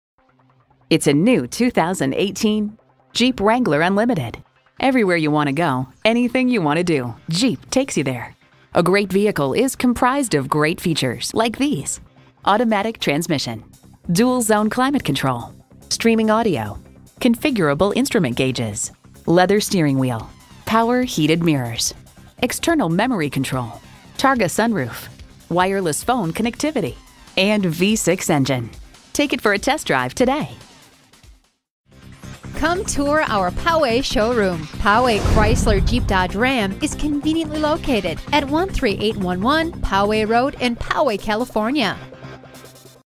New 2018 Jeep Wrangler Poway CA Escondido, CA
Year: 2018
Make: Jeep
Model: Wrangler
Trim: Unlimited Sport S
Engine: 3.6L 6-Cylinder
Transmission: 8-Speed Automatic
Color: Gray
Interior: Black
Stock #: C16665
VIN: 1C4HJXDG6JW298665
Crystal Metallic 2018 Jeep Wrangler Unlimited Sport S 4WD 8-Speed Automatic 3.6L 6-Cylinder ABS brakes, Compass, Electronic Stability Control, Low tire pressure warning, Traction control.

Hours